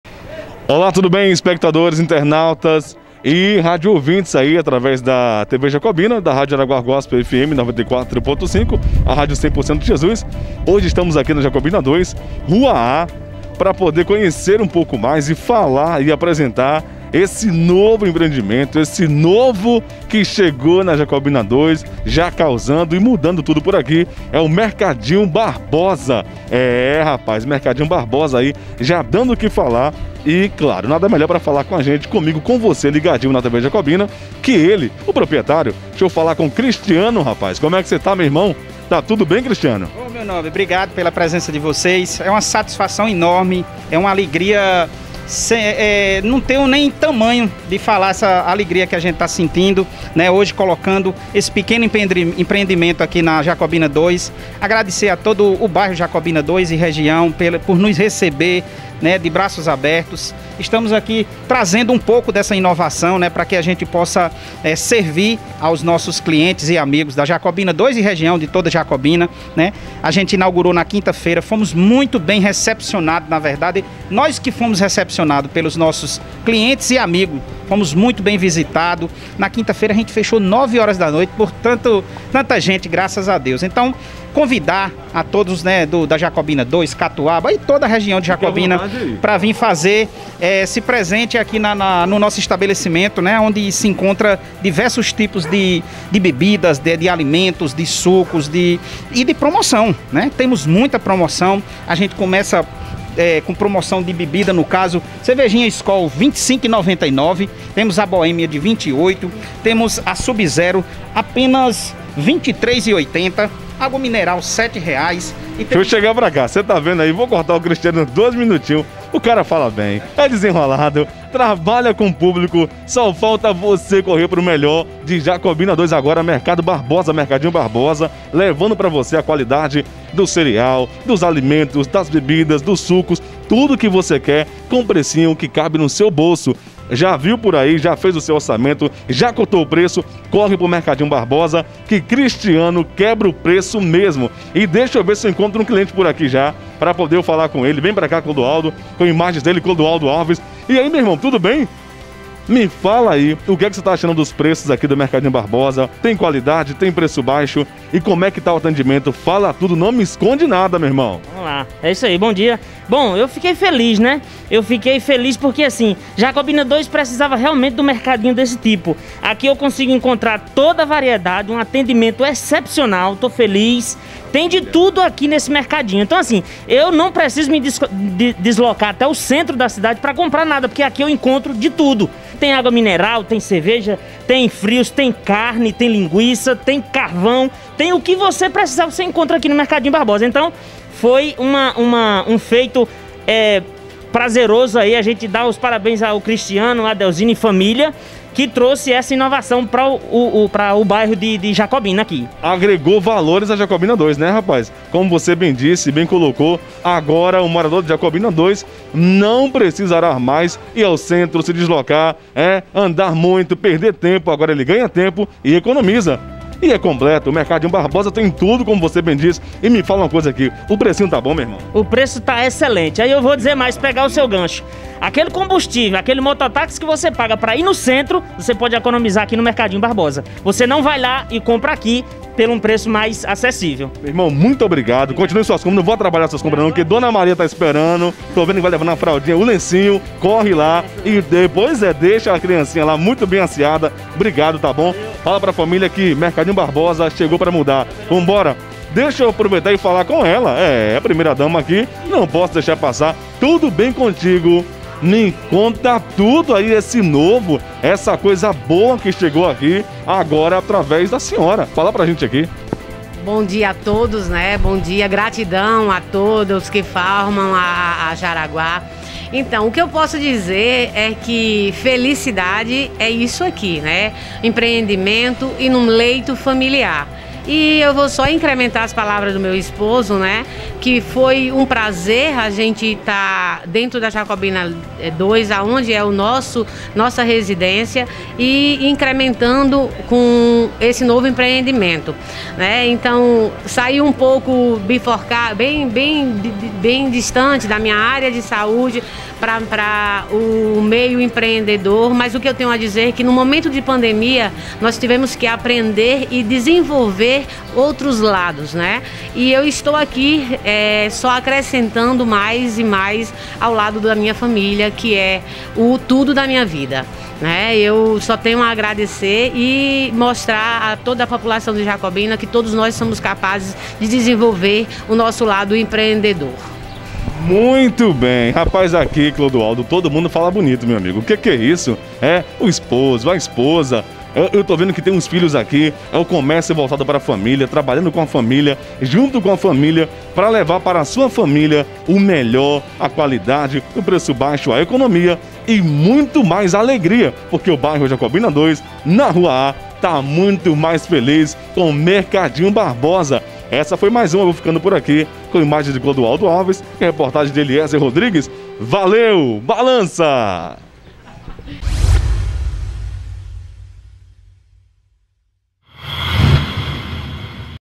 Mercadinho Barbosa é o mais novo empreendimento do bairro Jacobina II
O bairro Jacobina II acaba de receber seu mais novo empreendimento, o Mercadinho Oliveira, de propriedade do casal de empresários Cristiano e Adelzina Paiava.
Sucesso !